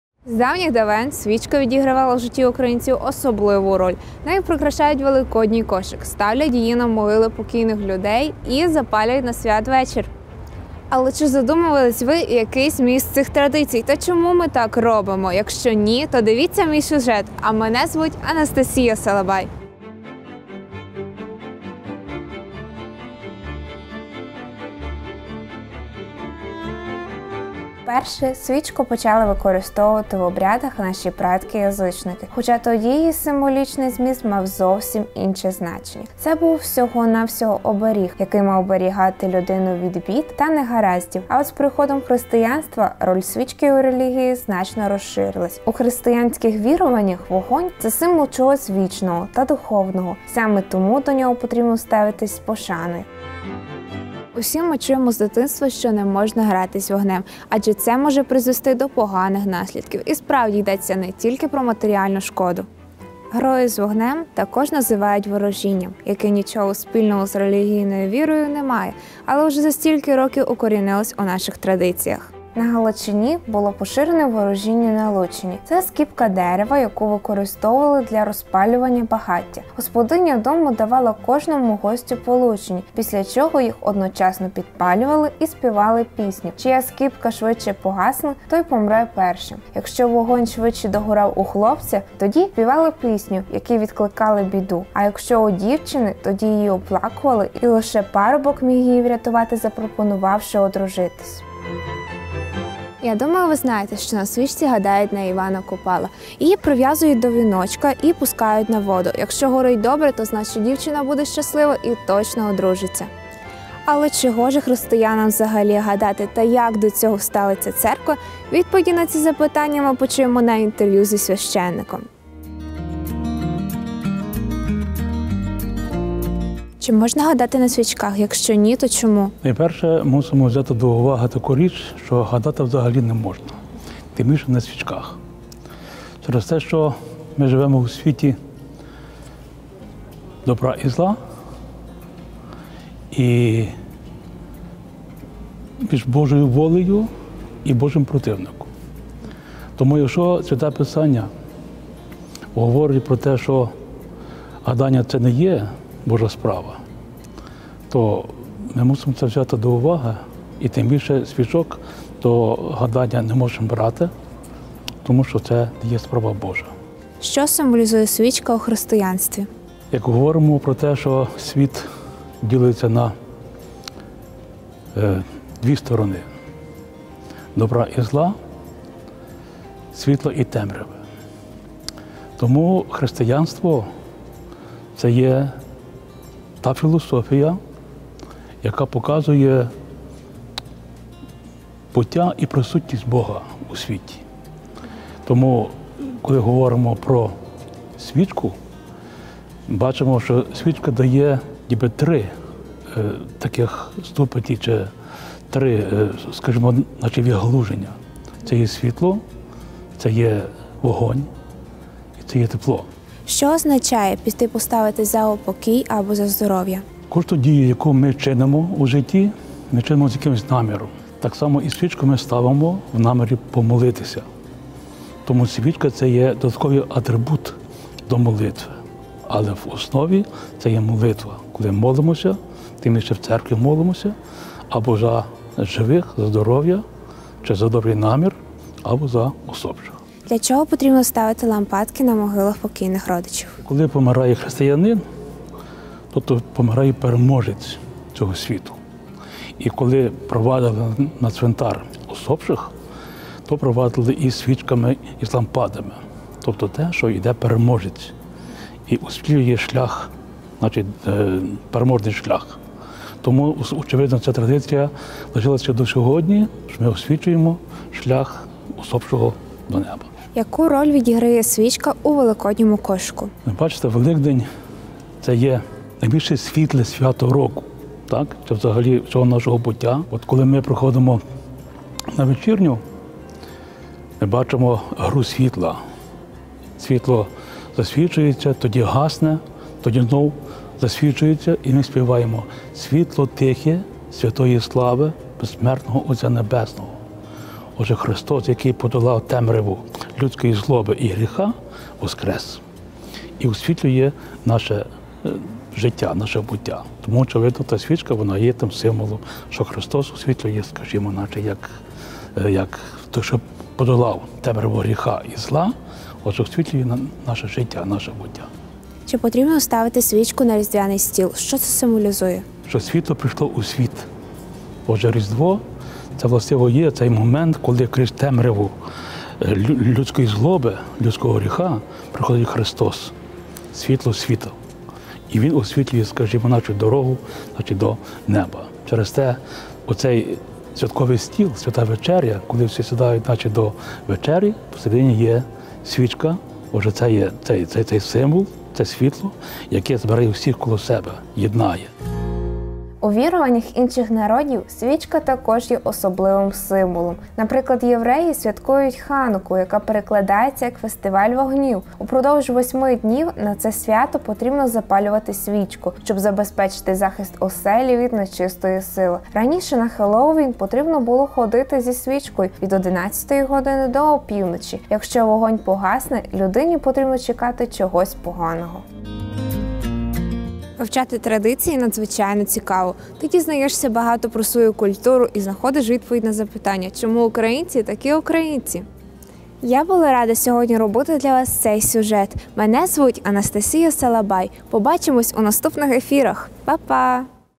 Приховані символи свічки. Анастасія Салабай / Тема: (Свічка)
Свічка – це предмет, який може освітити не лише зовнішній простір, але й внутрішній – вашу душу. Ця проста на вигляд річ приховує у собі силу-силенну символів, які пригадують нам про постійний вибір між добром і злом та між світлом і темрявою.
Детальніше розповість у сюжеті Анастасія Салабай.